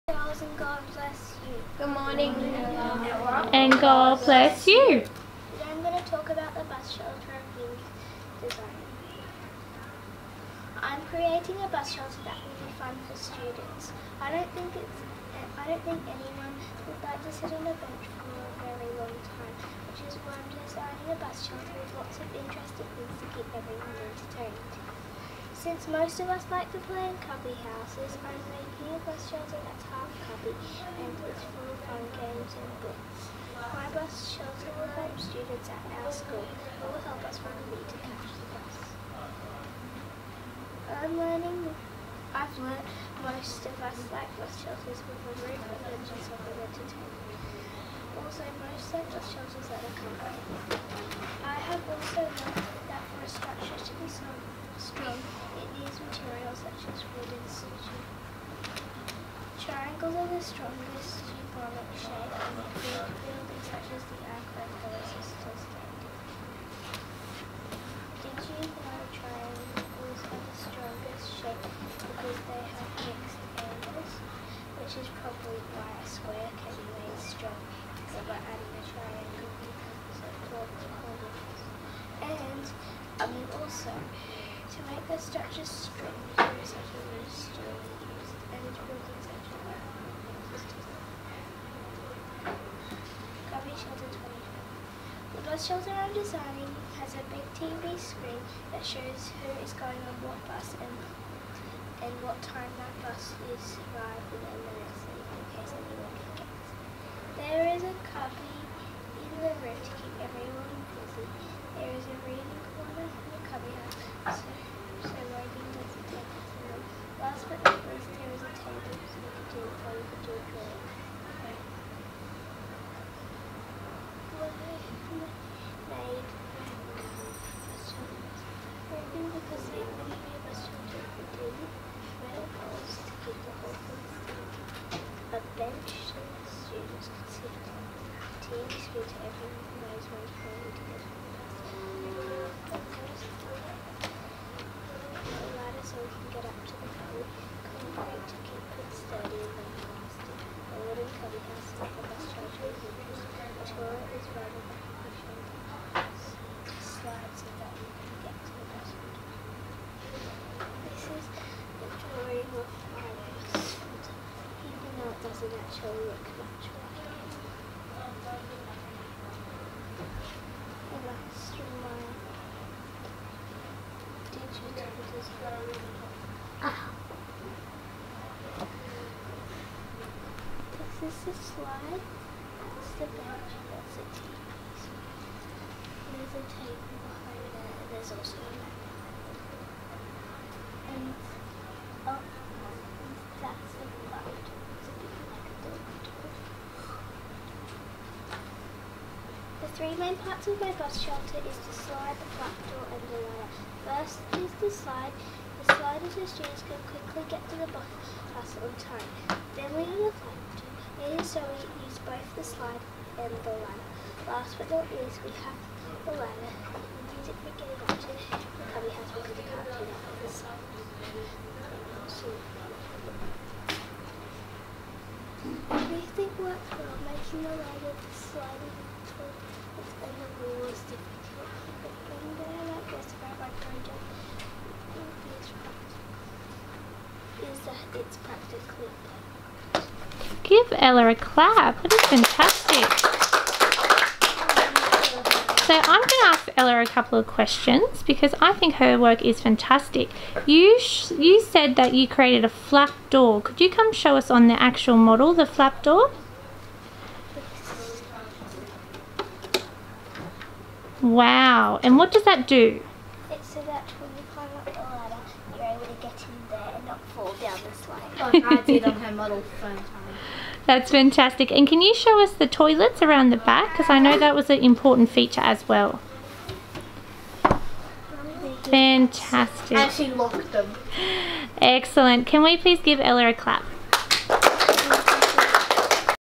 Year 3 Honours Presentation
Ella presents her idea for a bus shelter design.